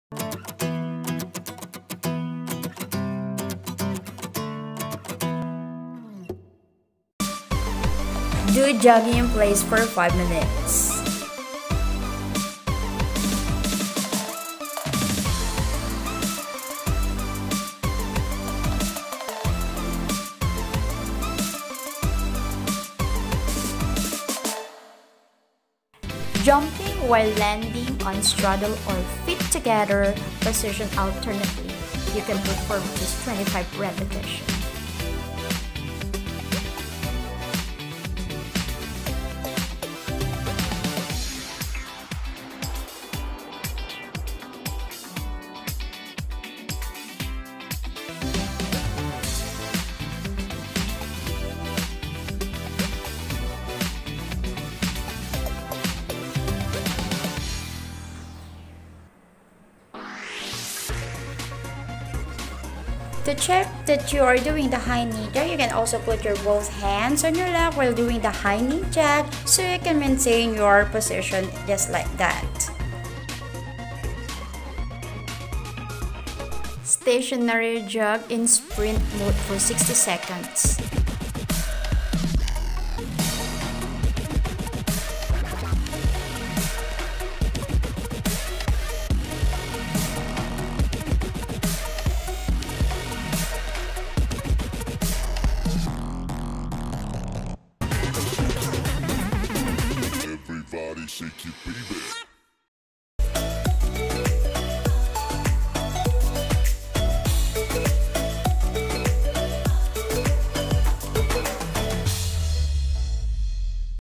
TRACK AND FIELD: Exercises and Drills for Running
Anybody can be a Runner. It is a matter of his/her priority. Meaning if you find yourself enjoying doing this kind of exercise, it can be hard sometimes but surely can have a development in your Physical Body.

 It is helpful especially this pandemic. Staying at home doing some exercise routine for 3-4 times a week is beneficial to your performance it can be physically or mentally. This video content can help you to accomplish your Running Routine.

Keep your mind and body away from any sickness by doing some exercise like this even inside your house.

Stay Healthy and Keep Safe!